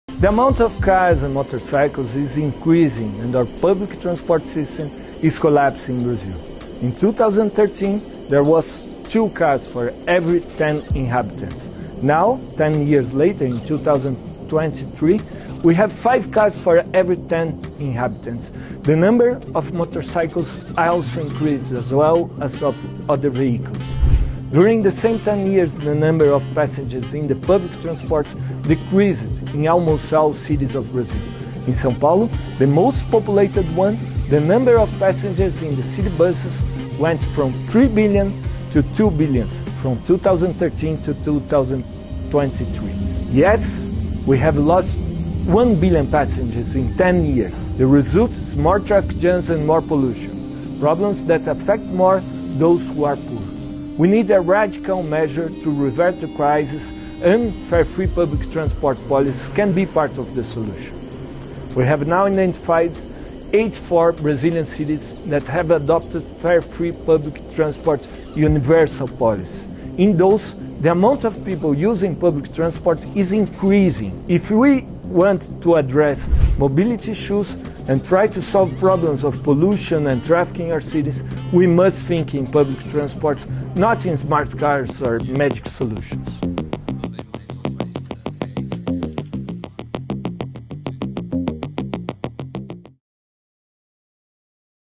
Right to Mobility For All
Mobility expert and activist for free public transport Daniel Santini of the RLS Sao Paulo talks about the right to mobility for all.